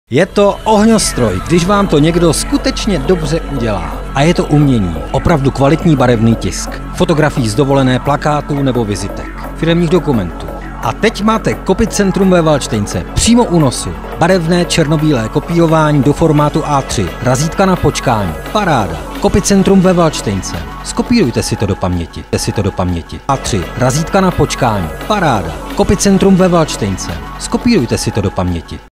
Copycentrum ve Valdštejnce - Kopírování a tisk Cheb
Kopírování barevné a černobílé, tisk z donesených fotek, tisk kalendářů, vizitek, letáků atd.